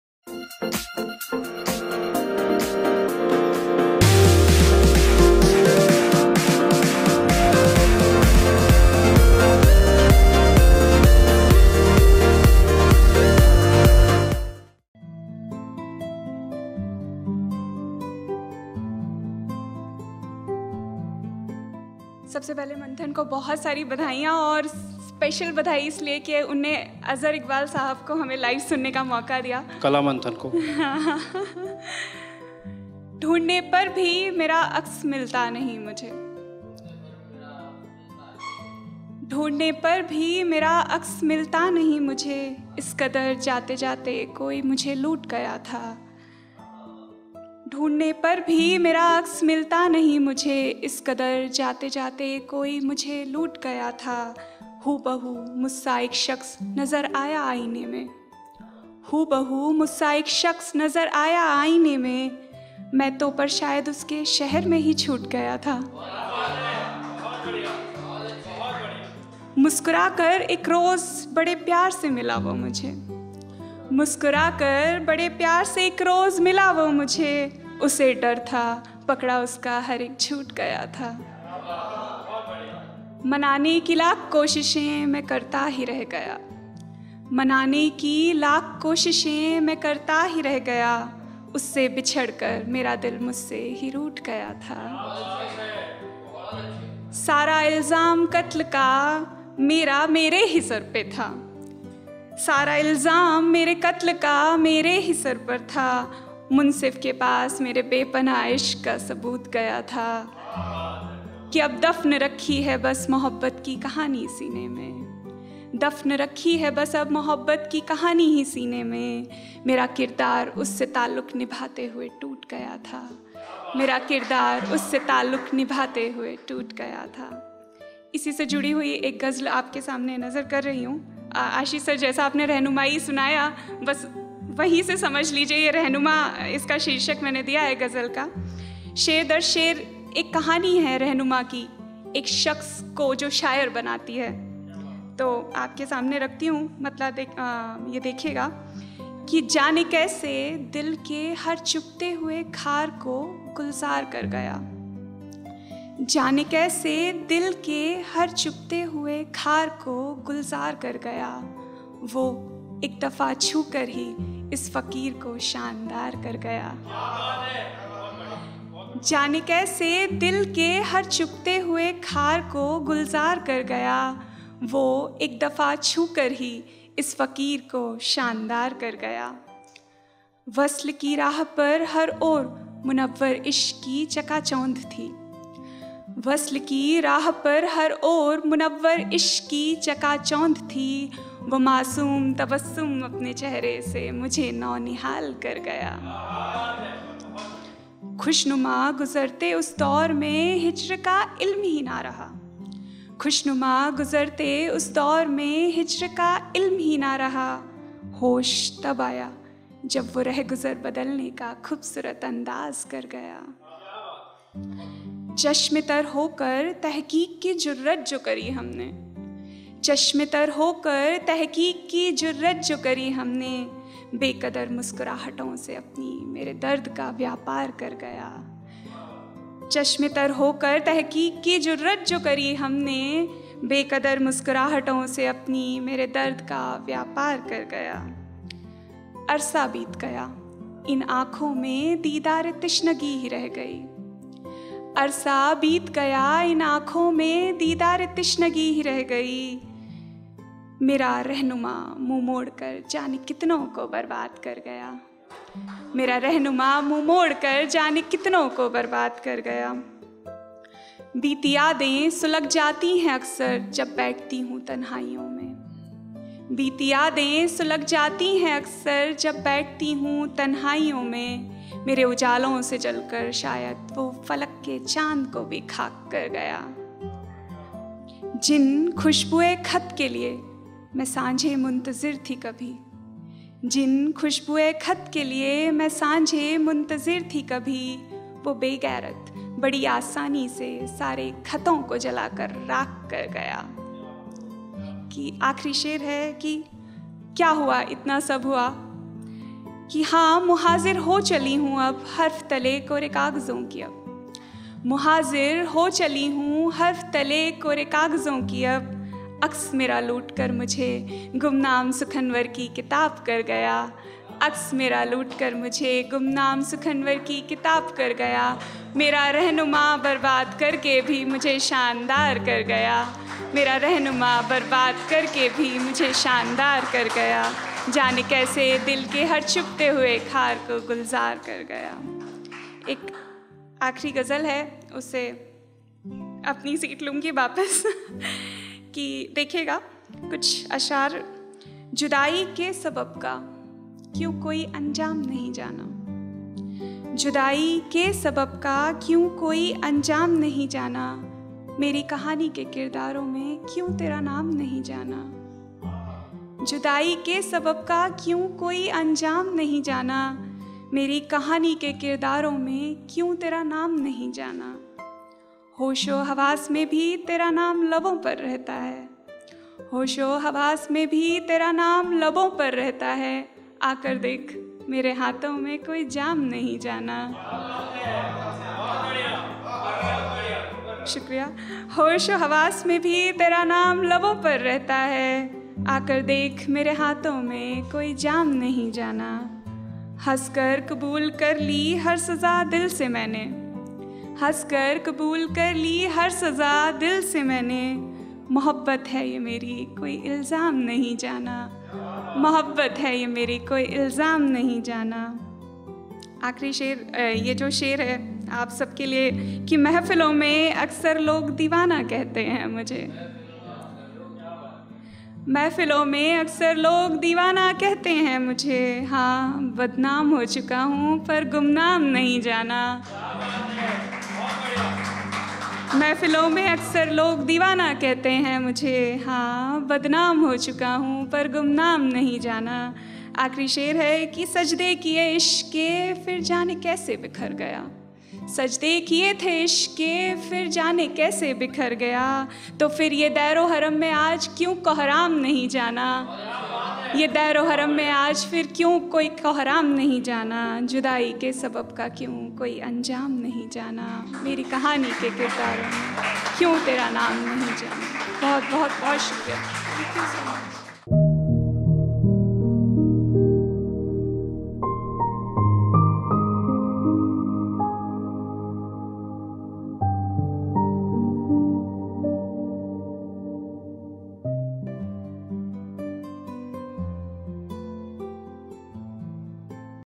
Pragya Nema || Kaavya Dhaara - Manthan Literature Festival
A talented young poetess with beautiful rendition.